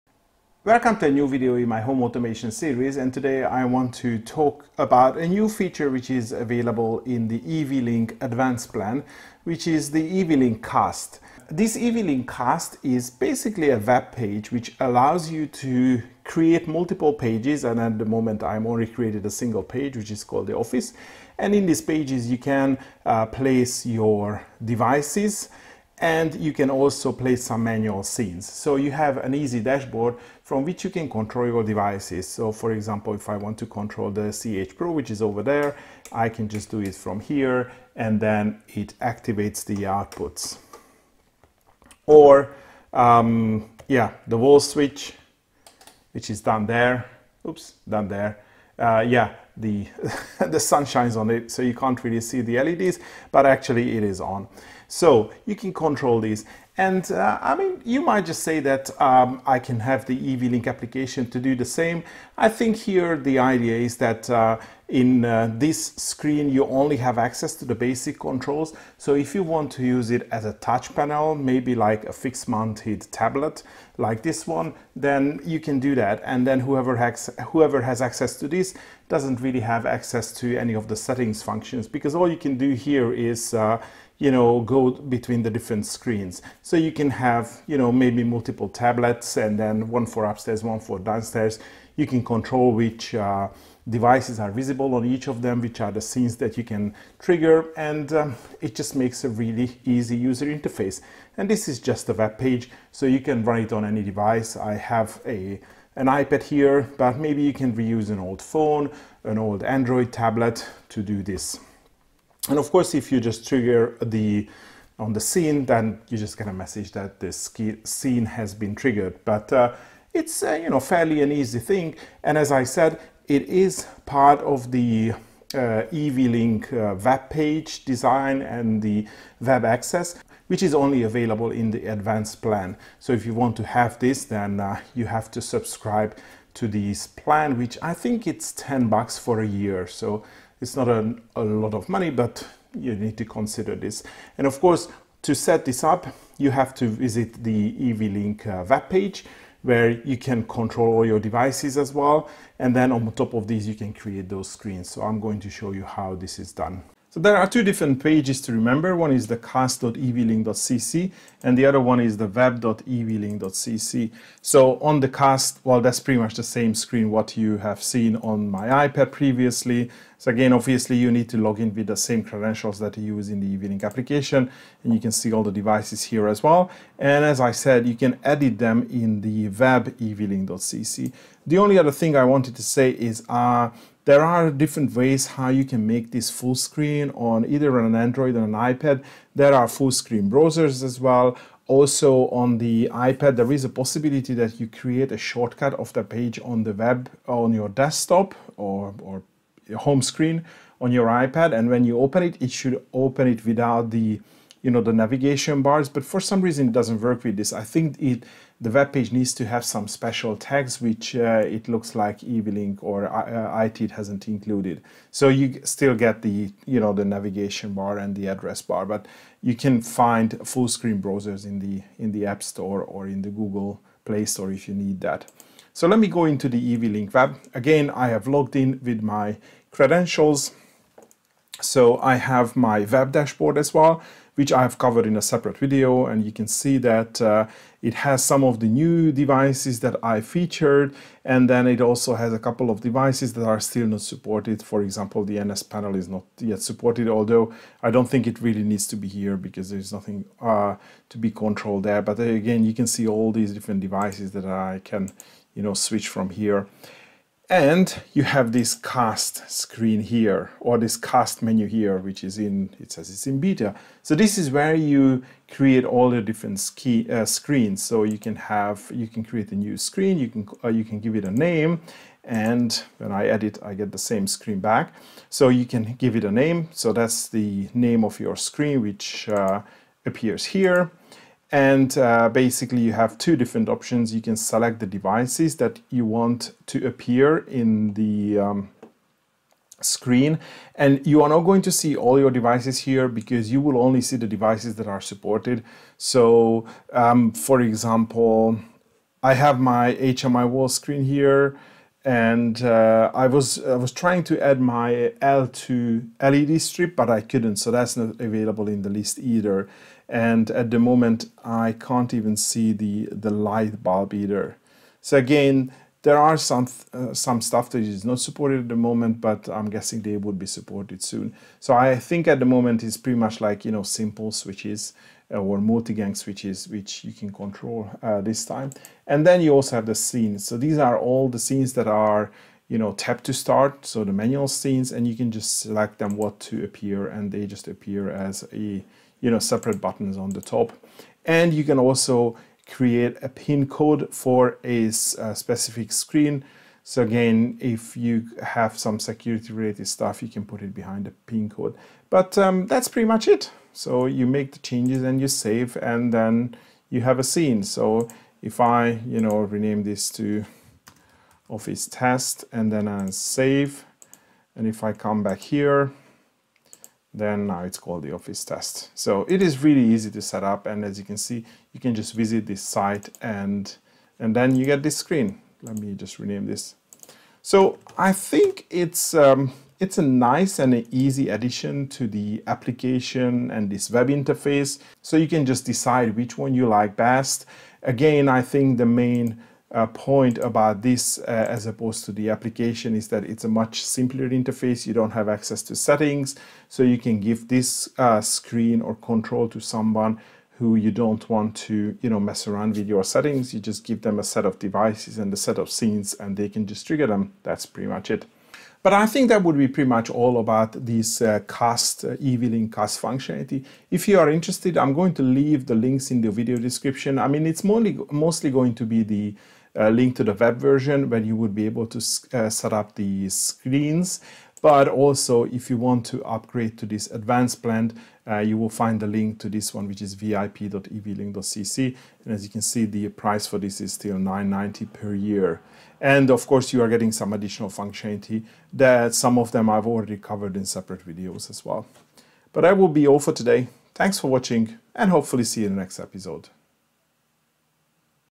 Touchscreen for Sonoff: eWeLink Cast
Turn your Apple or Android device to Sonoff touchscreen or touchpanel with a simple web interface.
This feature is only available for users paid for the eWeLink Advanced Plan.